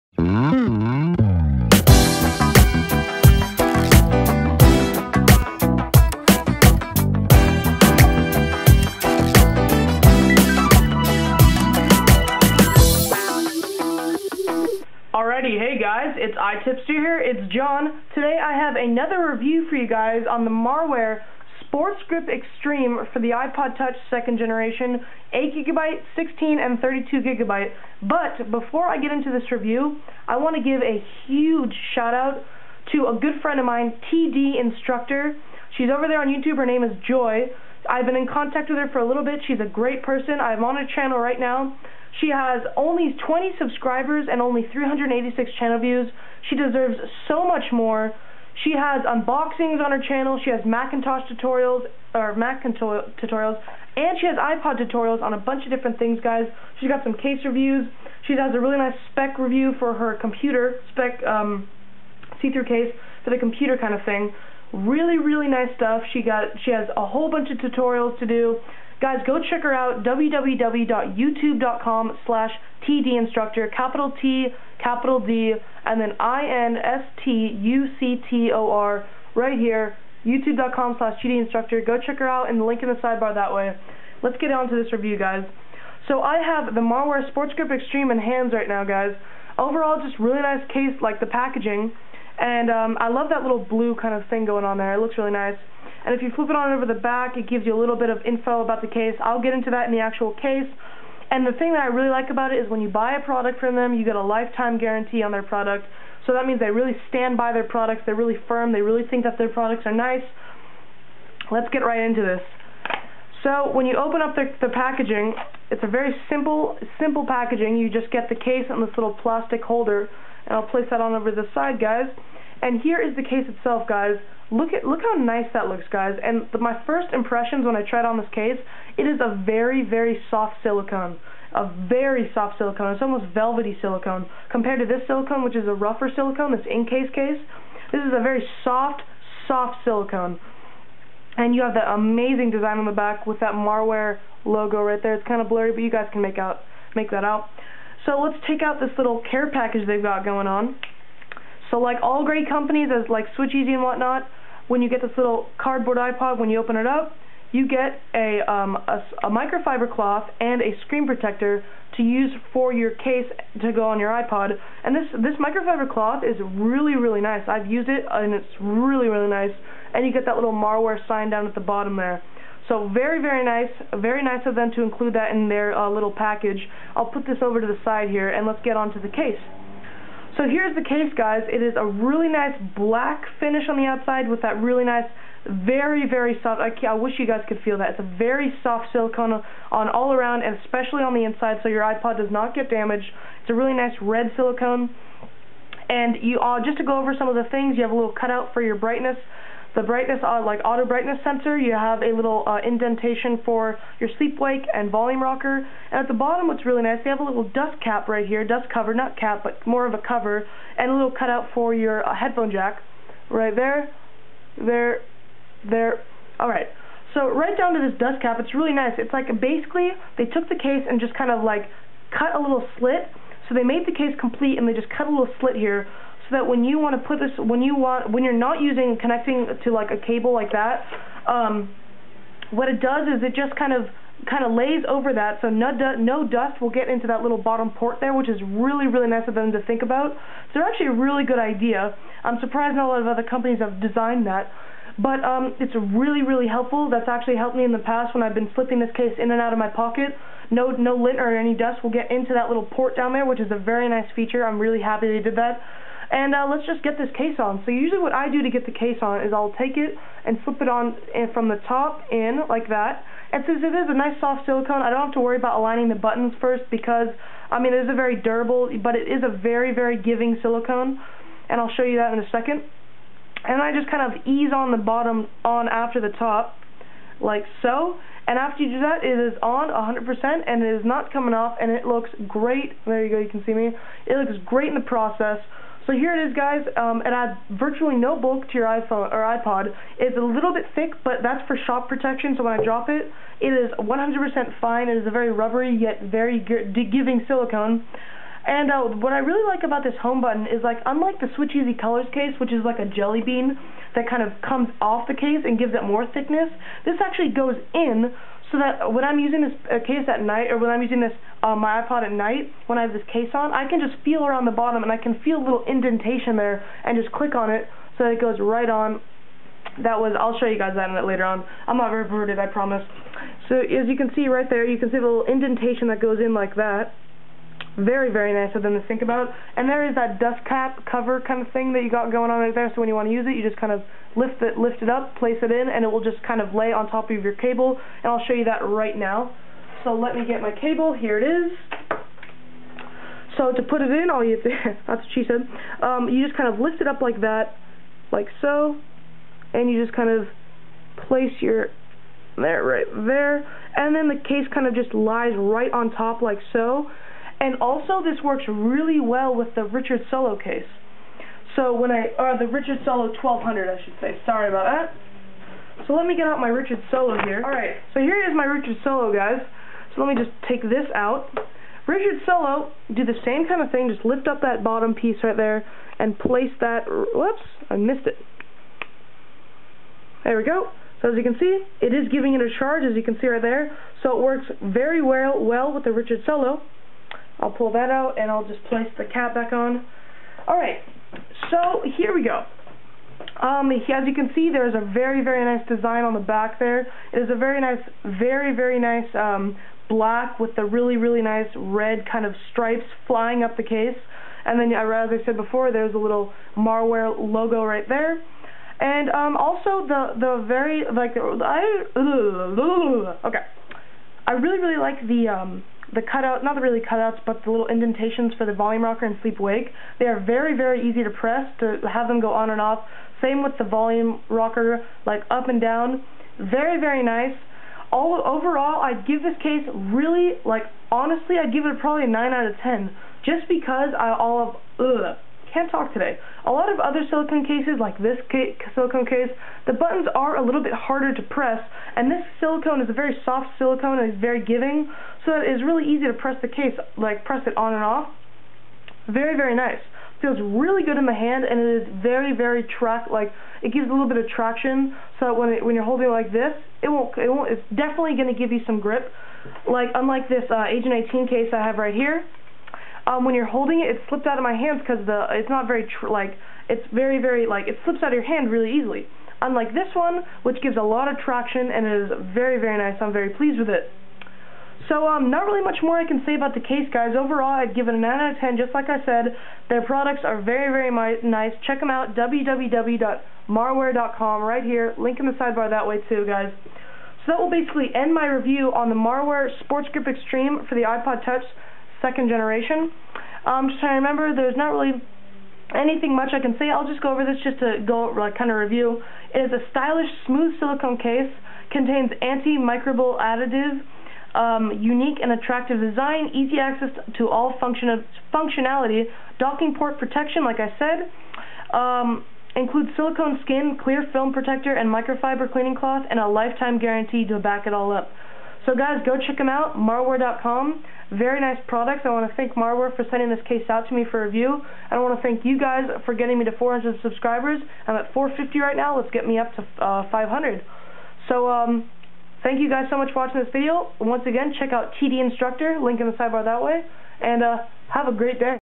Marware SportsGrip Extreme (for iPod Touch 2g)
A review on the Marware, Sports Grip Extreme, silicone case for the iPod Touch 2g. Enjoy!

Click Away!